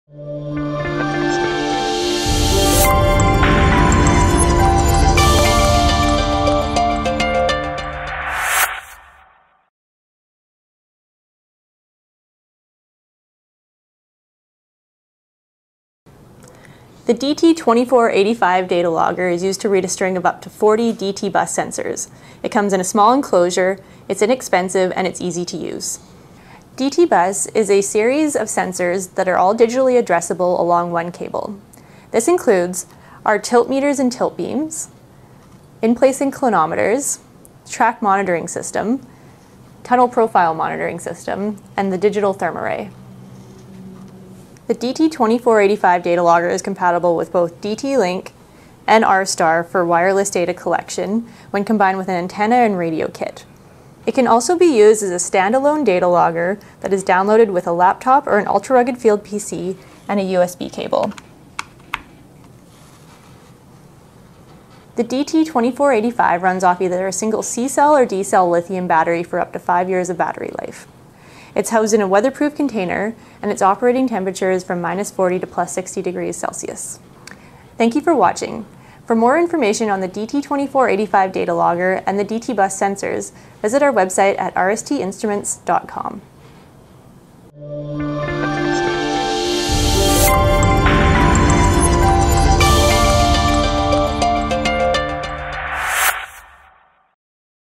DT2485 DT-BUS Data Logger
The DT2485 enables low cost, stand alone, and radio-linked data collection of In-Place Inclinometer Strings and other digital bus (DT-BUS) sensors.

It reads up to 40 DT-BUS sensors. DT-BUS is a series of digitally bussed sensors, all addressable, along one cable.

The DT2485 is compatible with DT Link and RSTAR for wireless data collection when combined with an antenna and radio kit. It can also collect data (non-wirelessly) through a USB with a laptop or Field PC.